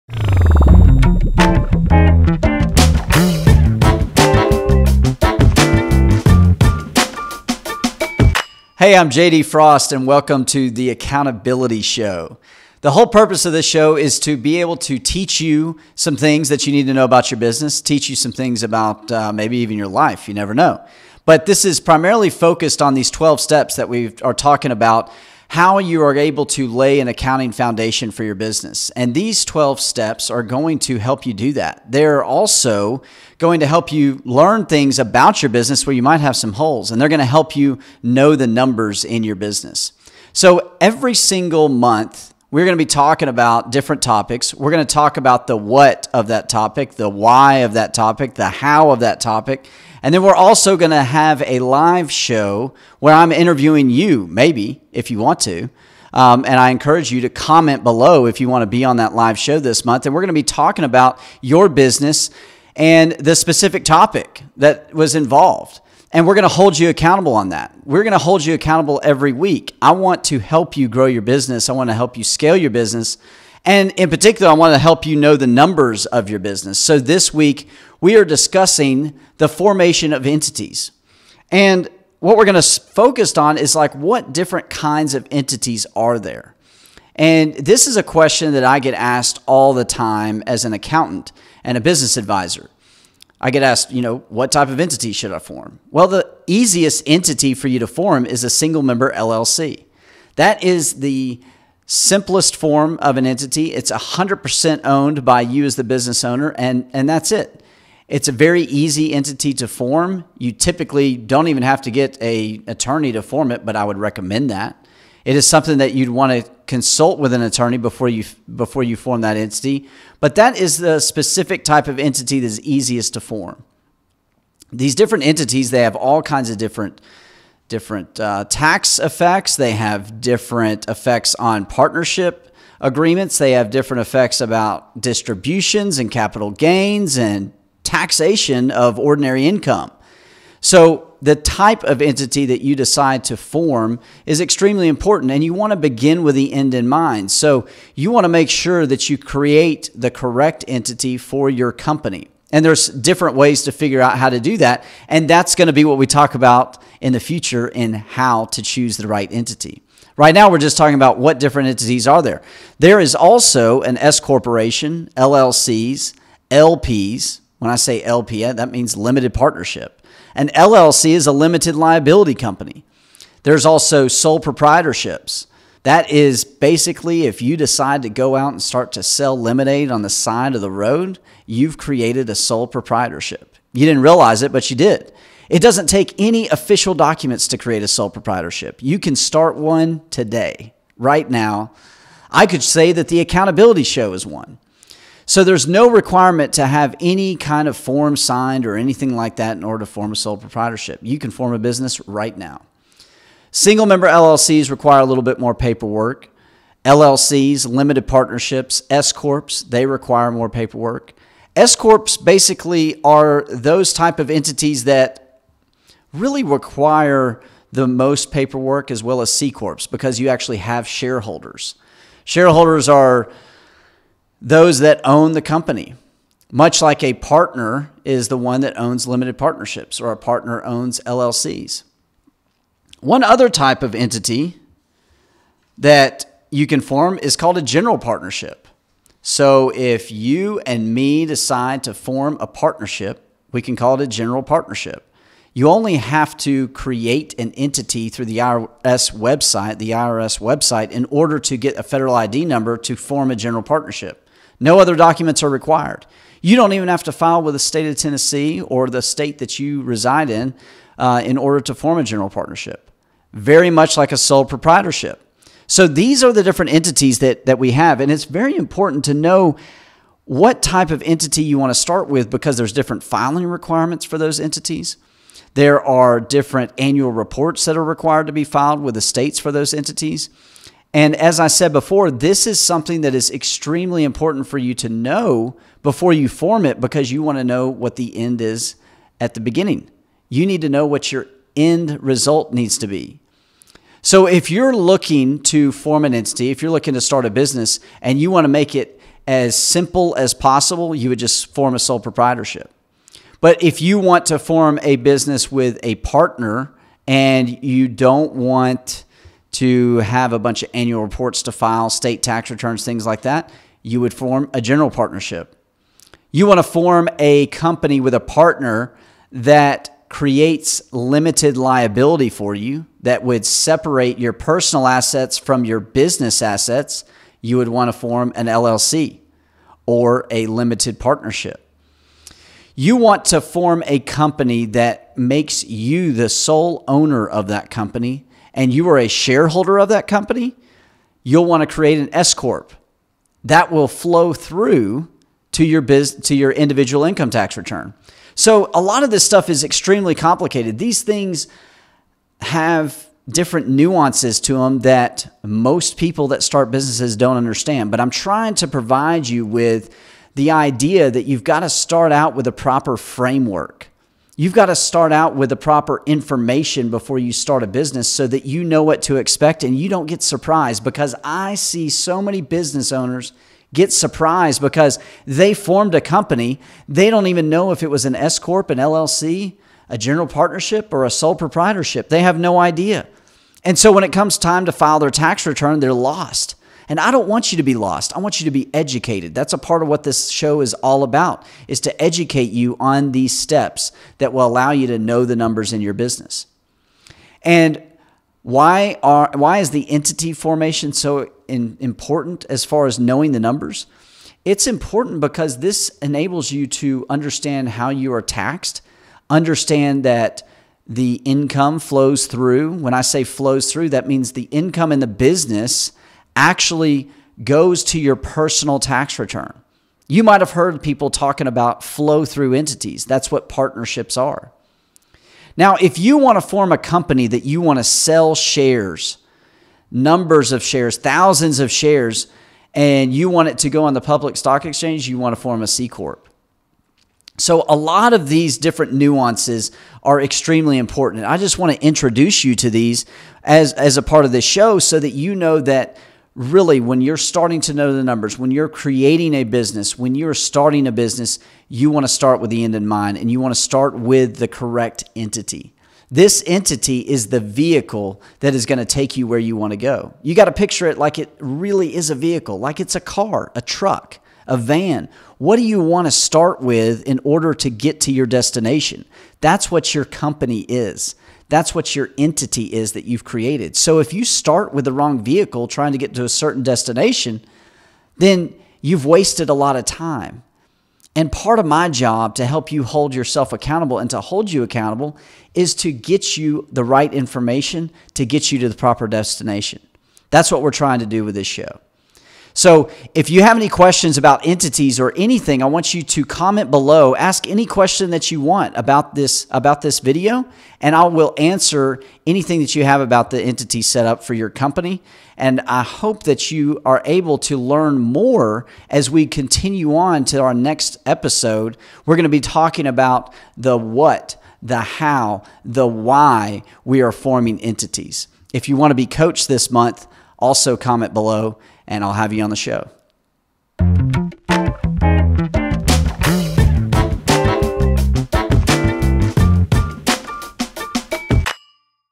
Episode 1: What Entity is Right For Your Business? • The Accountability Show with J.D. Frost
The Accountability Show - Episode 1

What entity is right for your business?

Welcome to the first episode in The Accountability Show series! J.D. Frost wants to help you hold yourself accountable in every aspect of life. The new series, The Accountability Show, discusses how to hold yourself accountable in your relationships with others, with yourself, and with your business.

This month’s theme on The Accountability Show with J.D Frost focuses on entity formation. In this episode, you will learn the different types of business entities and how to decide which entity is right for you and your business. Learning about the different filing requirements, annual reports, and tax implications of the entity you form is crucial to the success of your business and your end goal.

If you want to change your life or impact the lives of others, this show is for you!